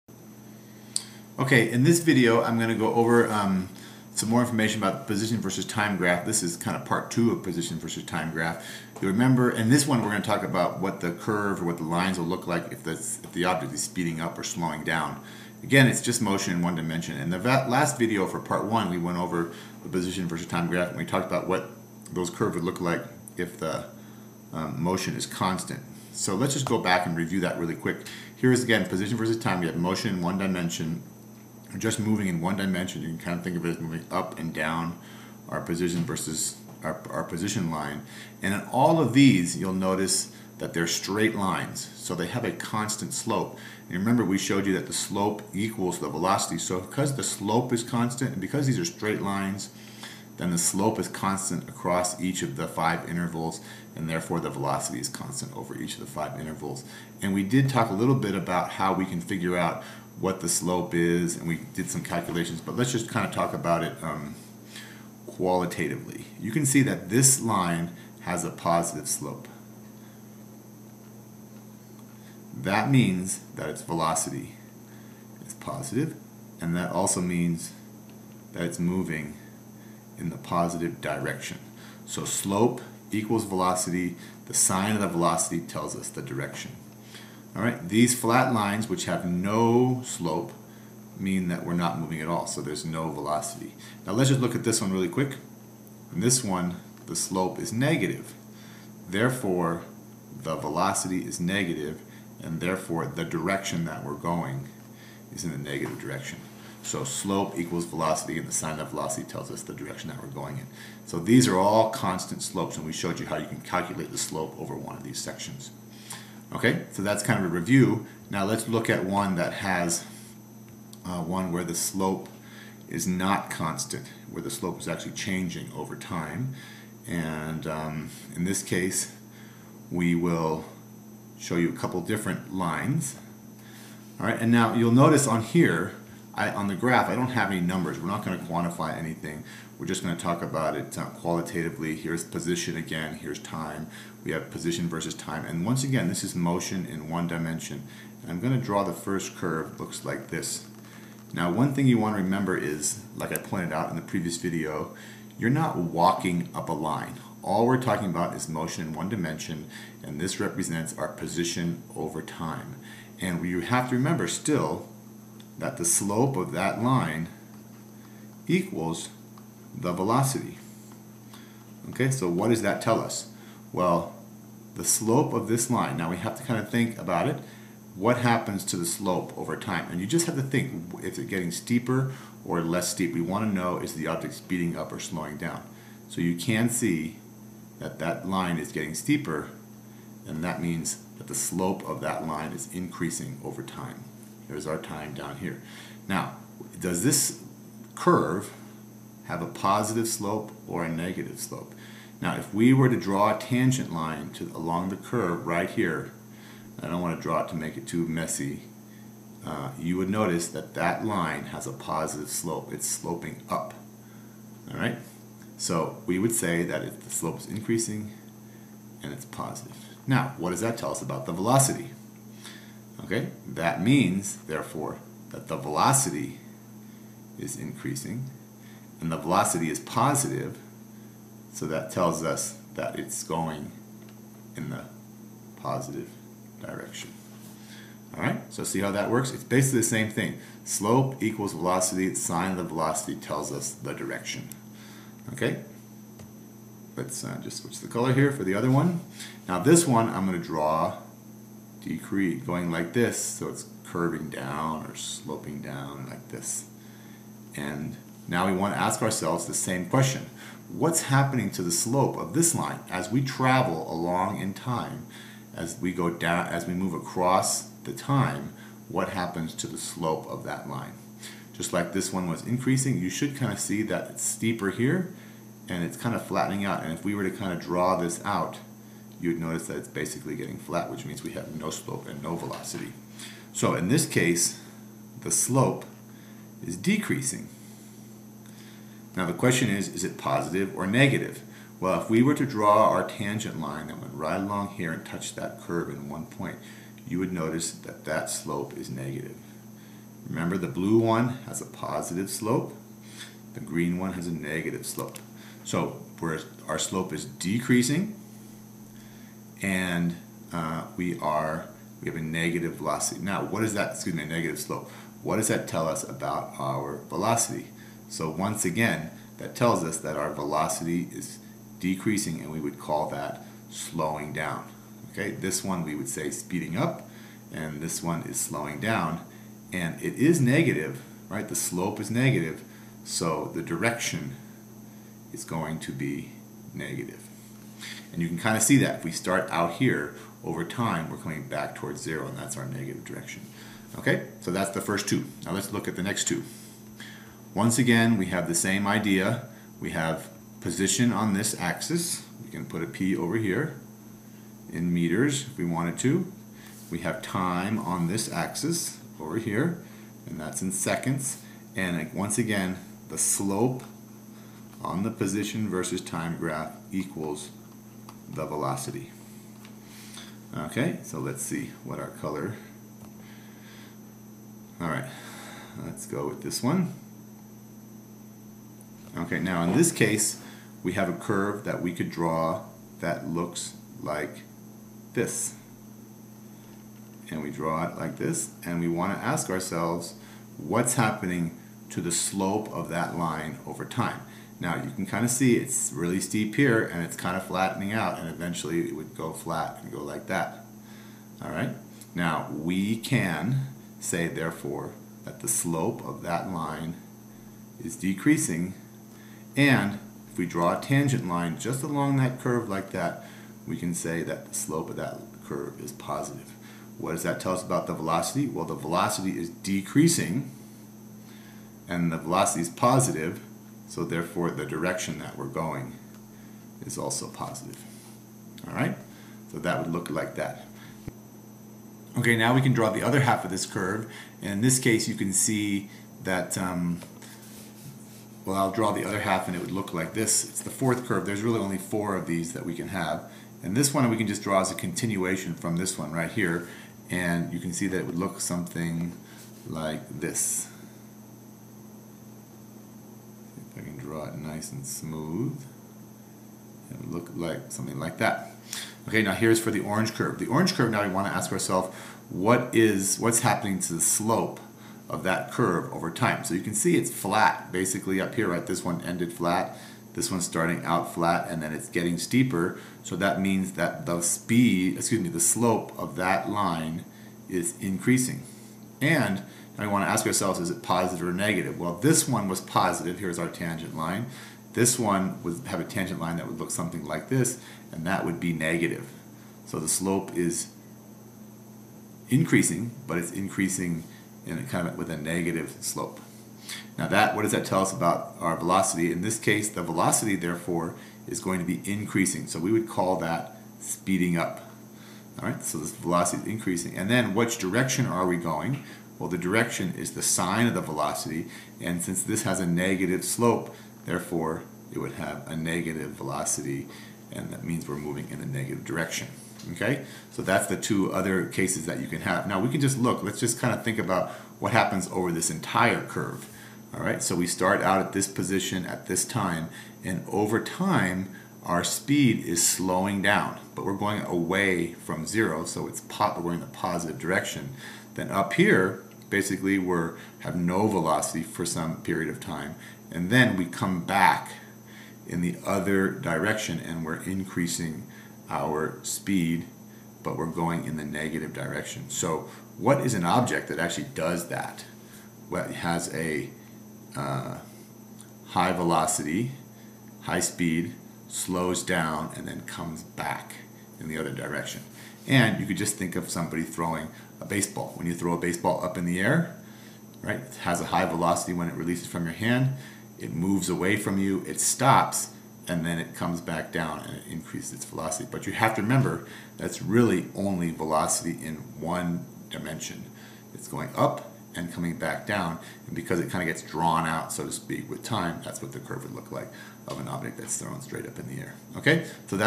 Motion Graphs (2 of 8) Position vs. Time Graph Part 2, Acceleration
Describes how to determine the slope, velocity and direction on a position vs. time graph for an object that is speed up or slowing down. You can see a listing of all my videos at my

Motion graphs are an excellent way to get an understanding of an objects motion over time. The slope of the line on the position vs time graph is equal to the velocity of the object. The slope of the line on the velocity vs time graph is equal to the acceleration of the object.